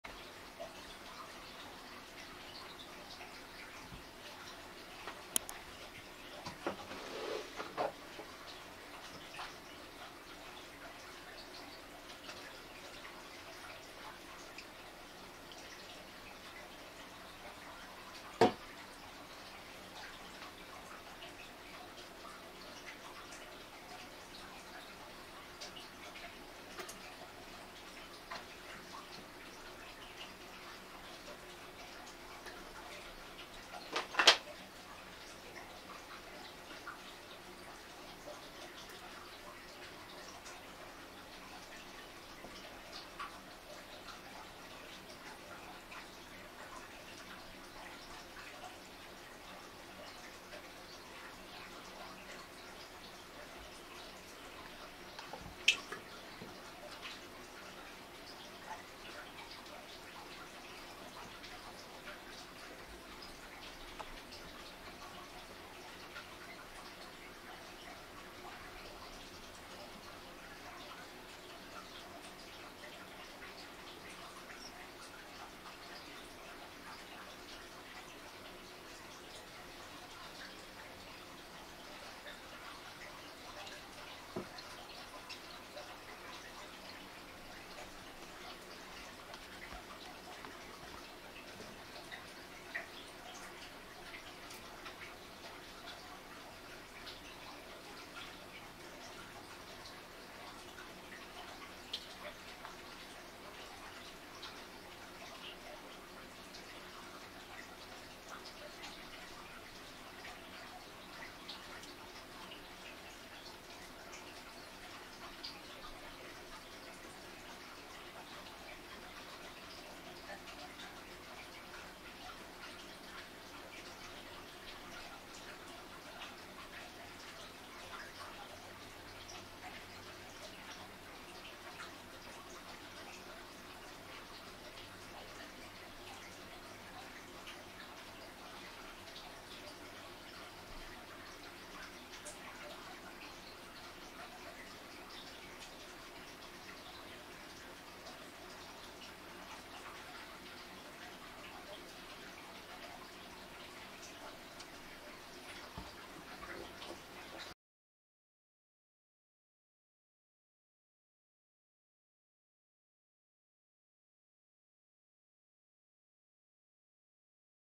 Giant African Bullfrog Eats - Part 1- Warning Live Feeding
This is a video documenting the live feeding habits and responses of The Giant African Bullfrog .